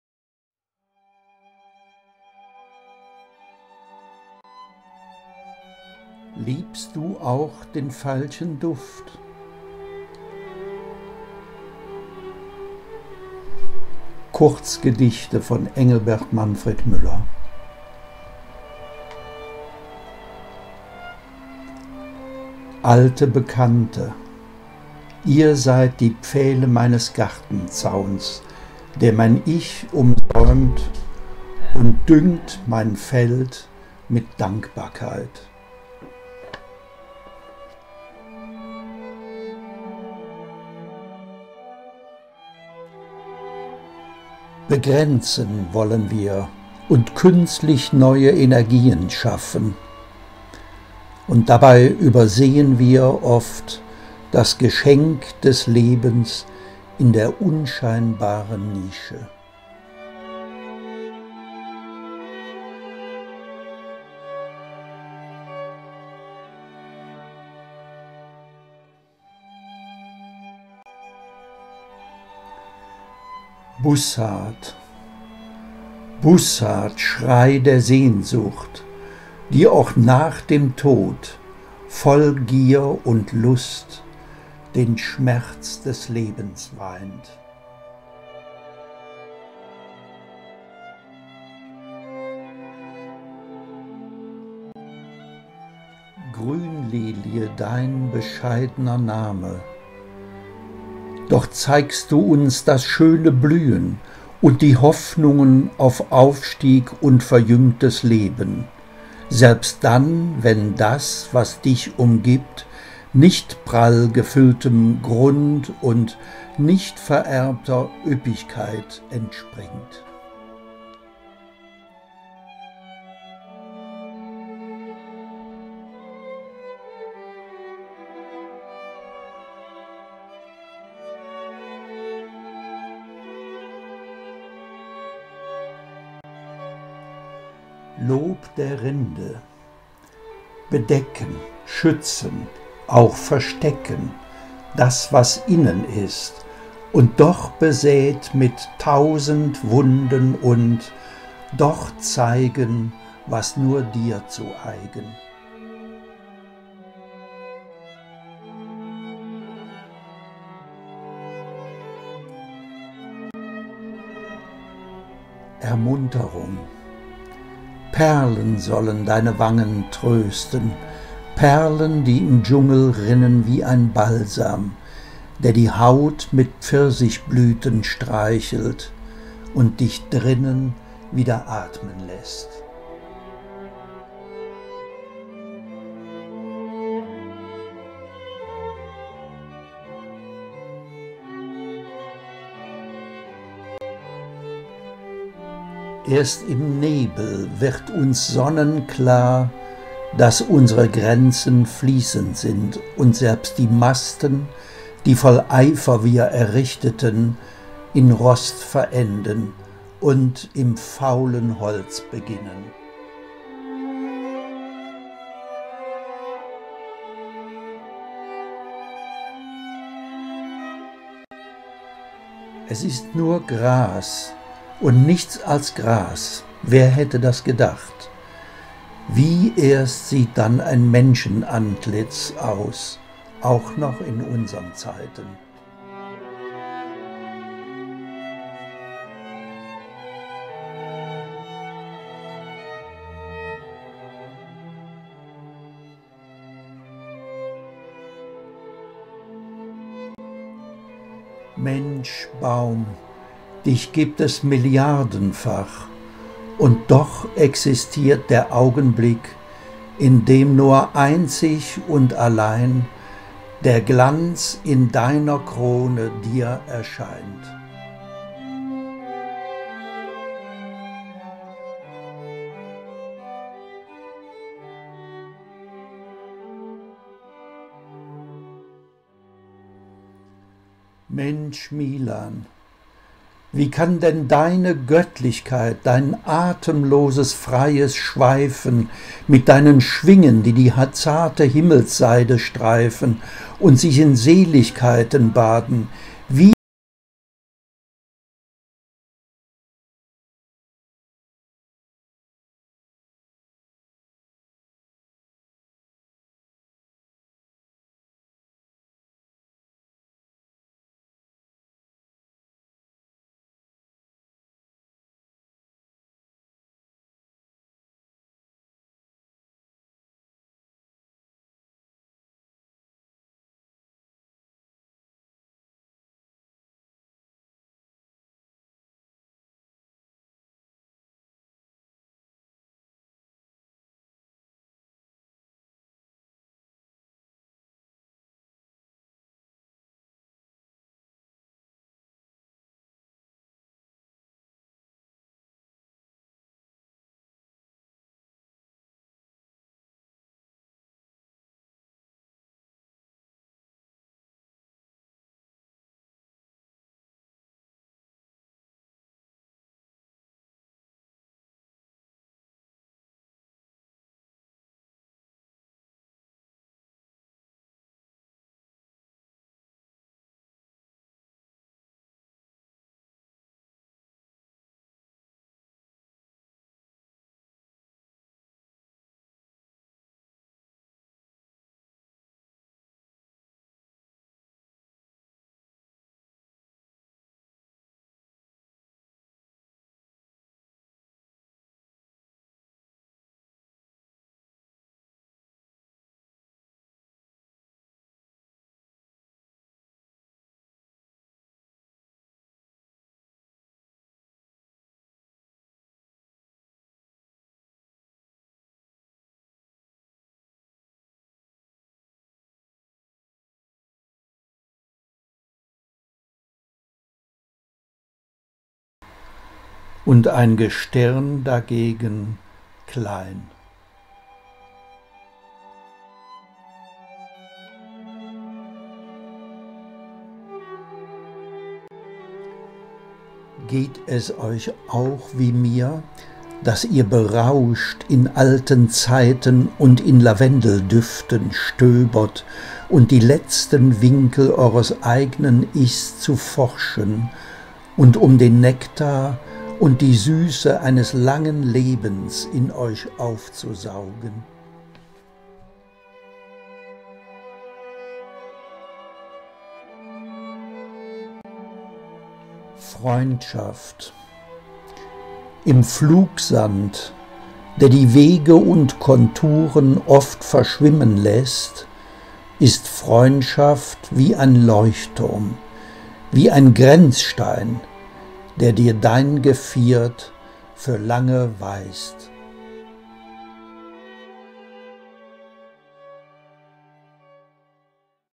Liebst du auch den Veilchenduft (gesprochen) Poesie für den Bildschirm
Freundschaft, alte Bekannte, Erinnerung sind einige der Themen der Kurzgedichte auf Bildern von Engelbert Manfred Müller